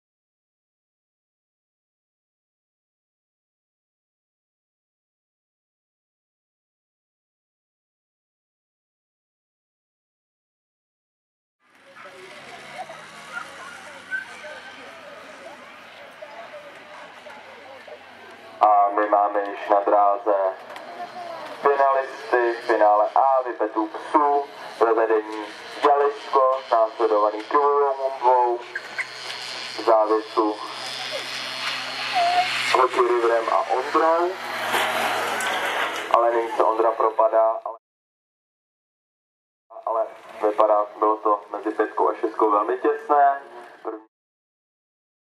Mistrovství Čech 2014, Mladá Boleslav 05.10.2014 - Whippet male FINAL A
Mistrovství Čech 2014, Mladá Boleslav - Whippet male FINAL A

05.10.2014

ALISCO Bohemia Snap Dog
KIVUVA MUMBWA Bohemia Snap Dog
HOKEE RIVER du Tepee d'Amour
KIPKETER Bohemia Snap Dog
RAINBOW RUNNER Aviaticus
ONDRA Vlapan